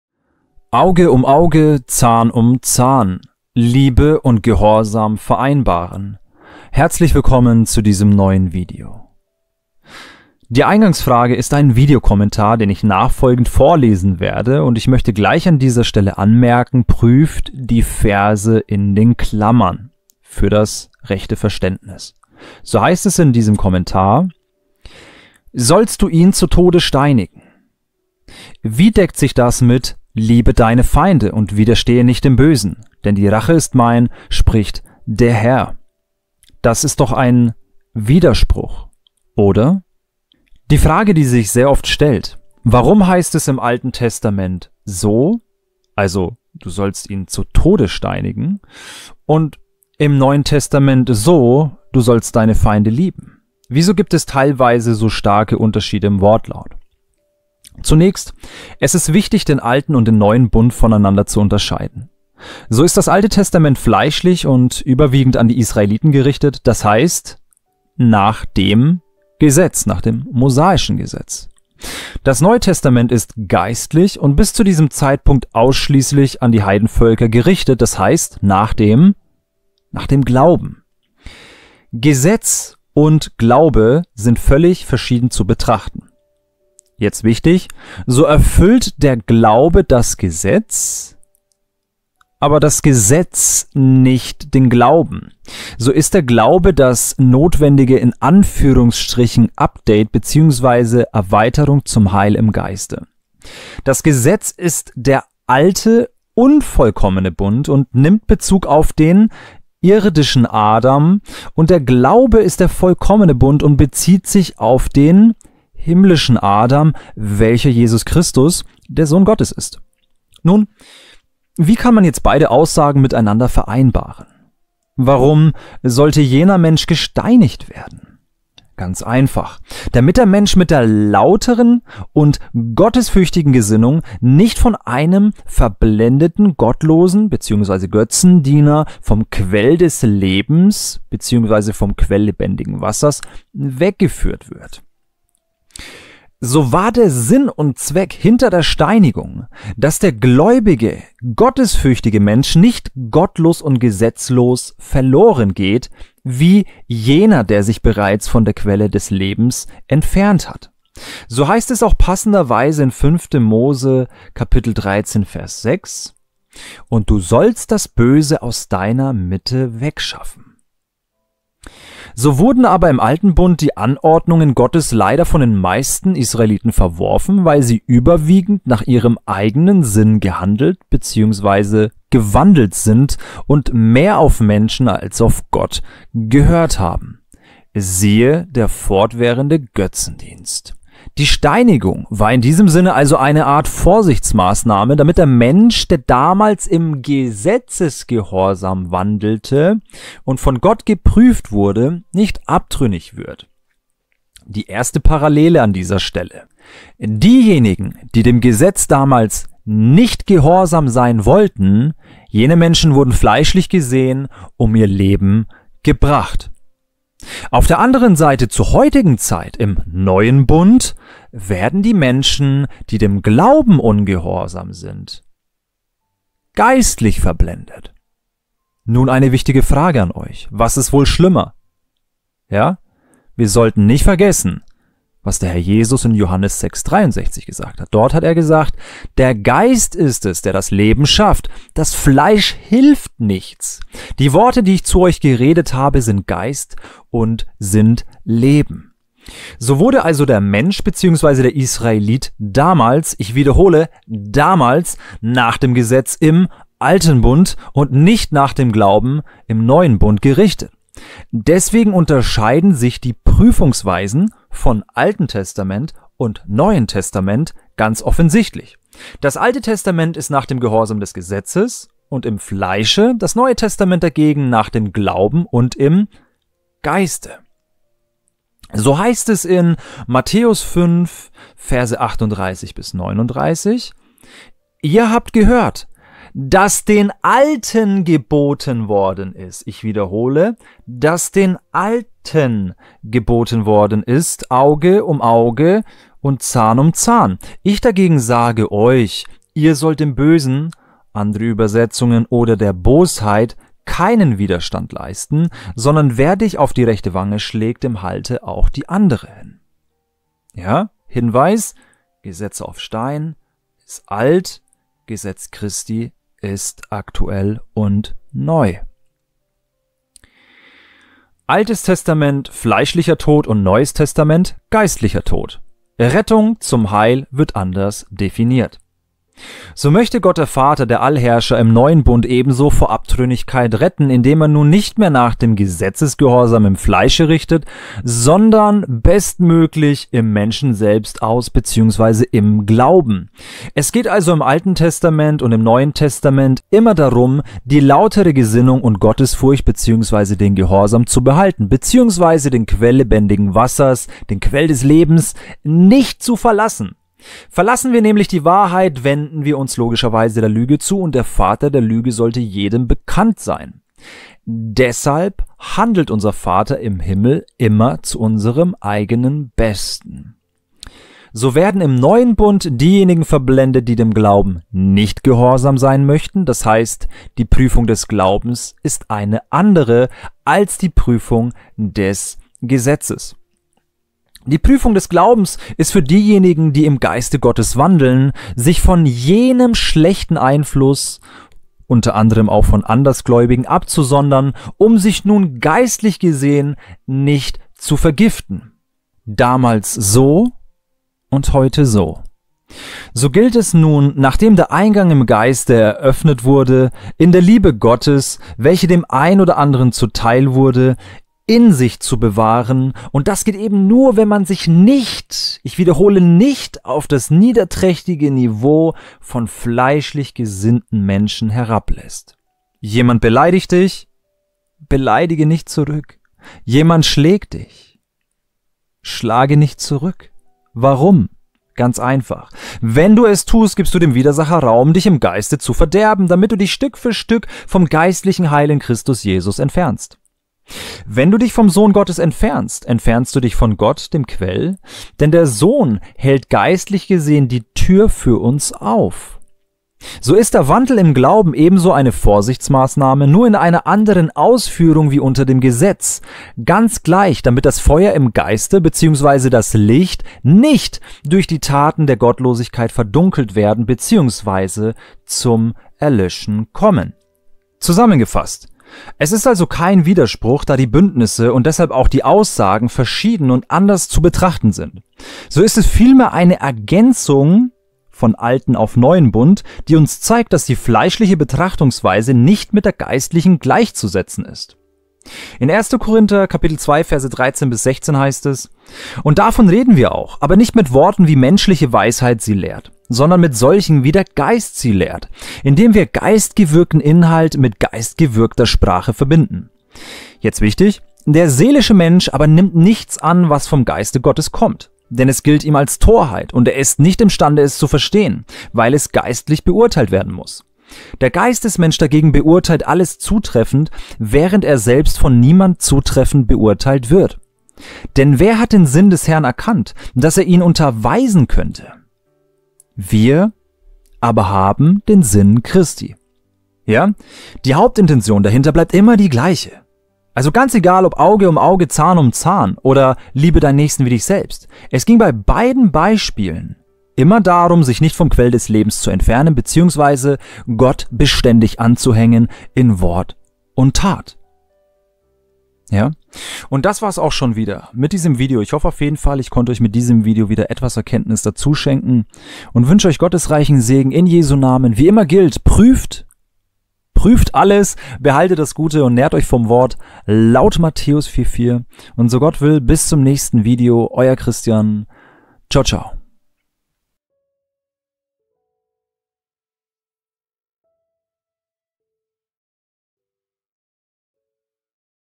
Auge um Auge oder - Liebe deine Feinde? Gehorsam ist Liebe...
1Kor 9,21 für die (Heiden), die das Gesetz nicht haben, zu einem Manne, der ohne das Gesetz lebt – obgleich ich nicht ohne Gottes Gesetz lebe, vielmehr dem Gesetz Christi unterworfen bin –, um die, welche das Gesetz nicht haben, zu gewinnen;
Gal 6,2 Traget einer des andern Lasten, so werdet ihr dadurch das Gesetz Christi erfüllen.